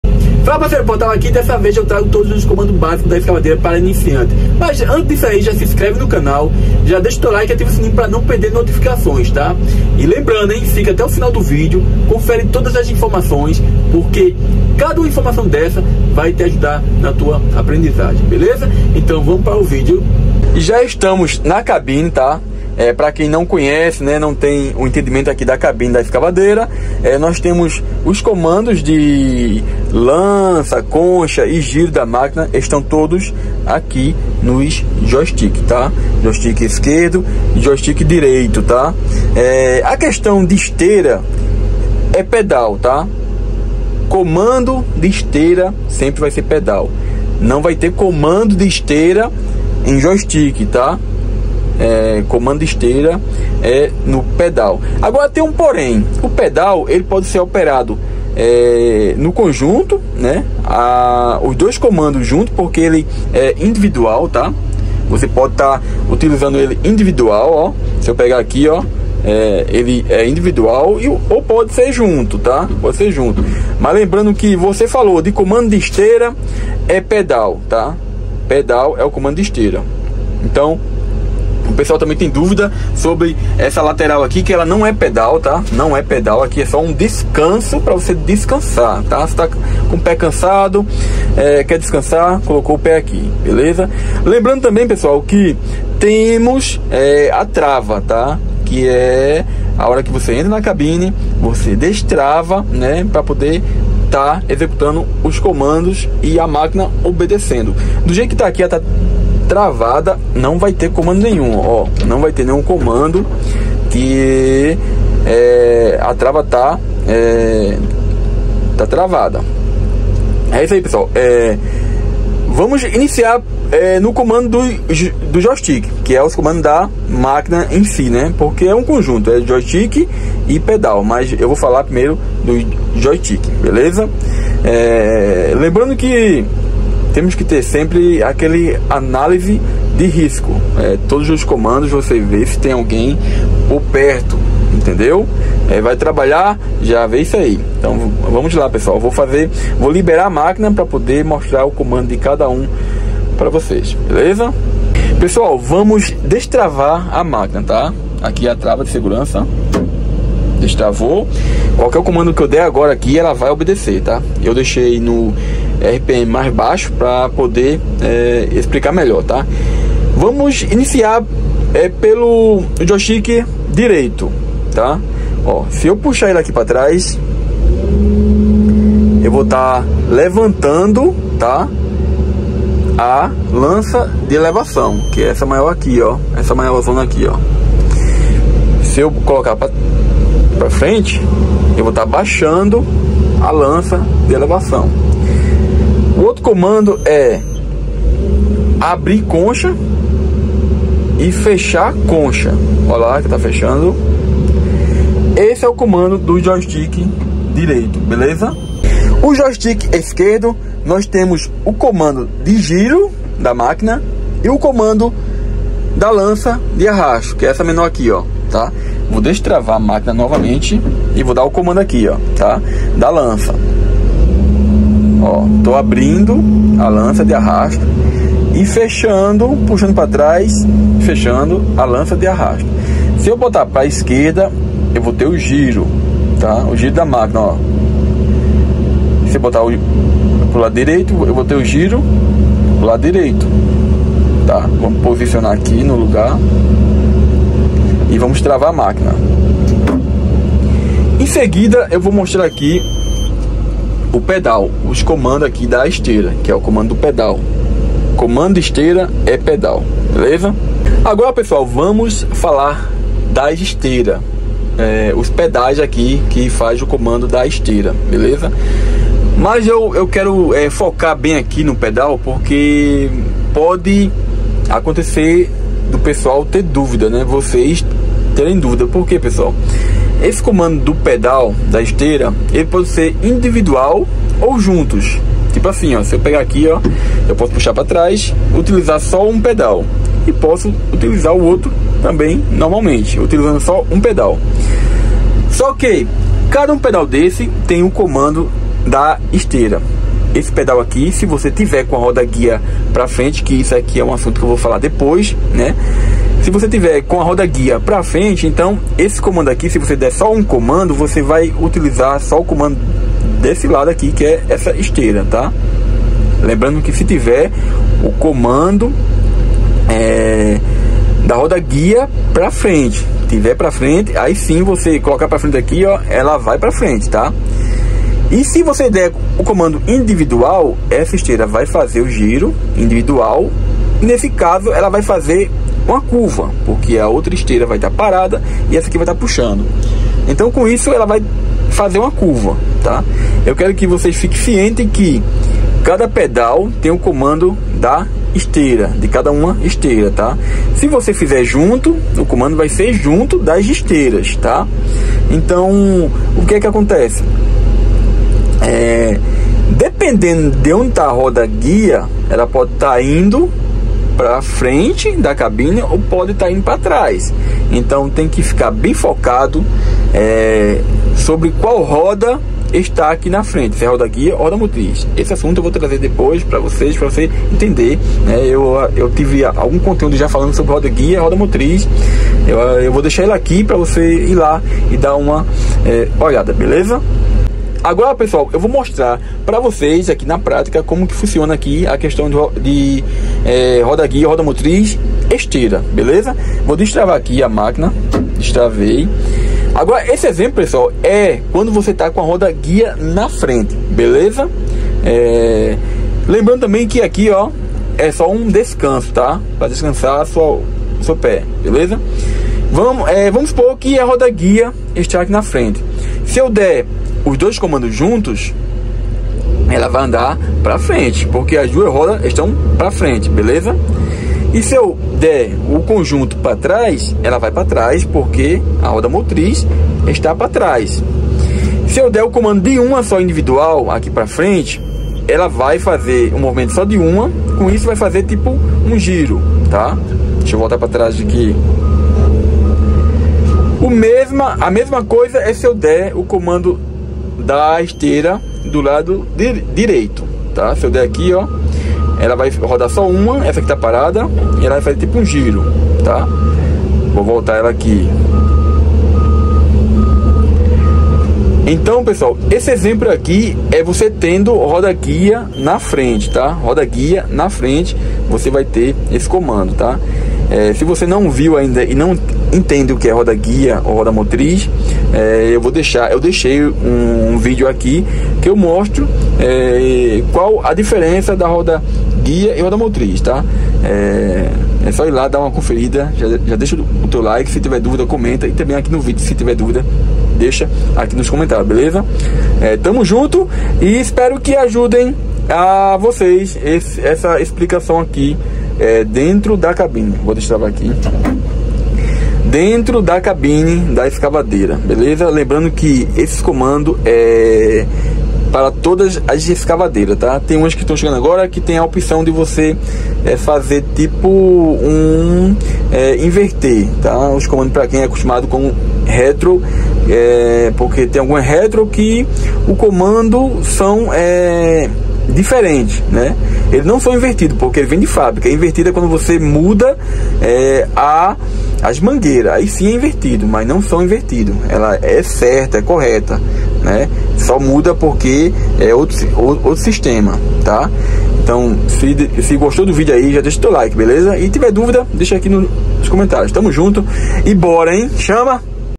Passei tudo que o INICIANTE PRECISA saber dos comandos ESCAVADEIRA HIDRAULILCA
Passei tudo que o INICIANTE PRECISA saber dos comandos ESCAVADEIRA HIDRAULILCA, PASSO A PASSO sem enrolação.

 Fala operador dde máquinas e iniciante em operação,nesse vídeo estou mostrando todos os comandos basico de operação da escavadeira para iniciante, comandos de pedal e de joystik!